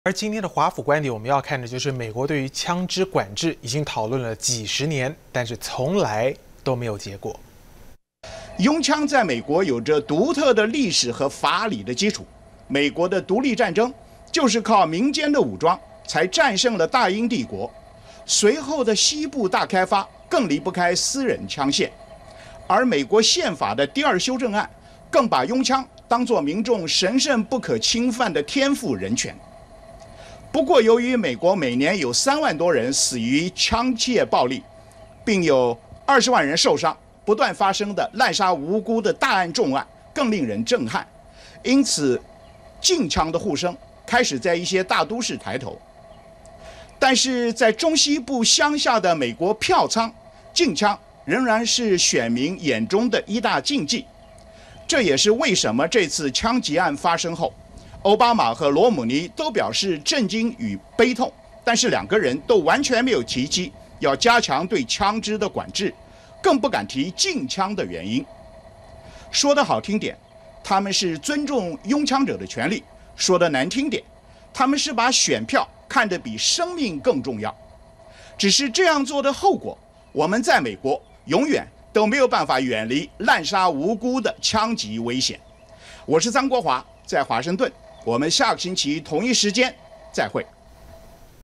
【中天】7/24 科羅拉多槍擊案 再掀起槍枝合法問題
今天的華府觀點要看的是美國對於槍枝管制，討論了幾十年，但是從來沒有結果。